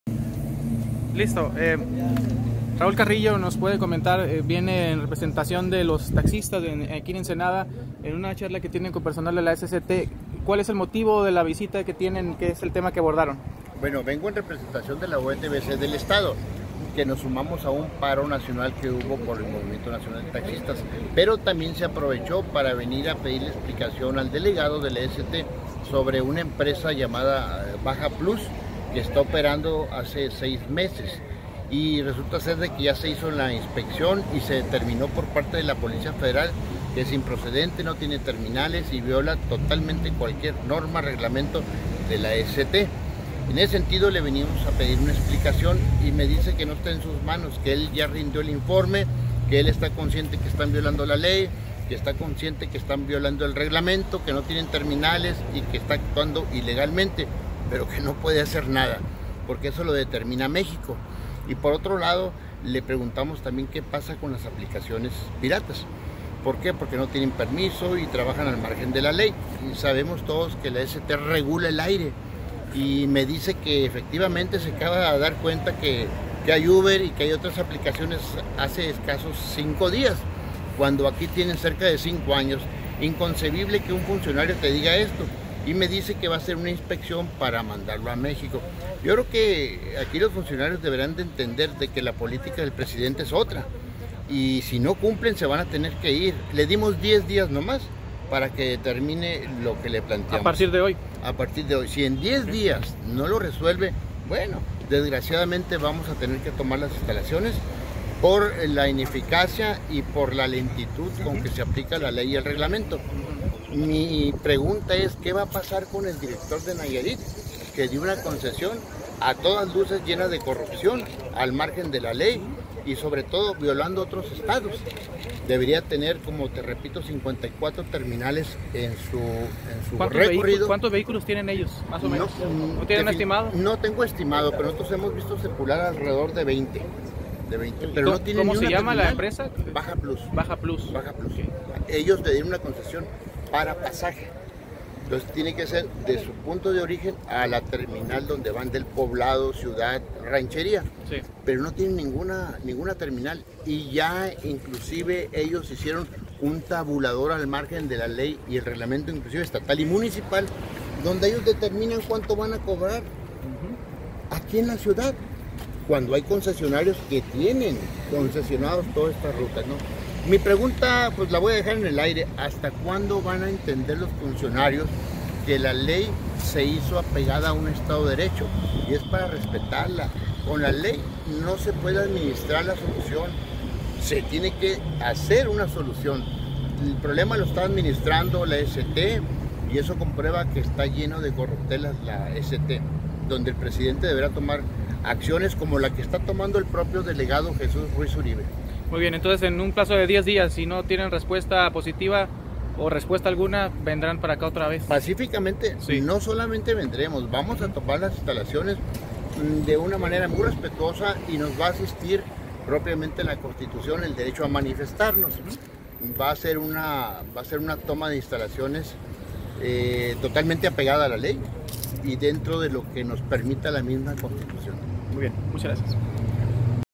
Entrevista con Raúl Carrillo líder de la UETBC
Entrevista con Raúl Carrillo líder de la Unión Estatal de Transportistas de Baja California.
Se manifiestan ante la Secretaría de Comunicaciones y Transportes, inconformes por empresa de transporte que opera de manera irregular denominada Baja Plus; y aplicaciones privadas como UBER que dicen son aplicaciones piratas.

Video: Ricardo Meza Godoy / plex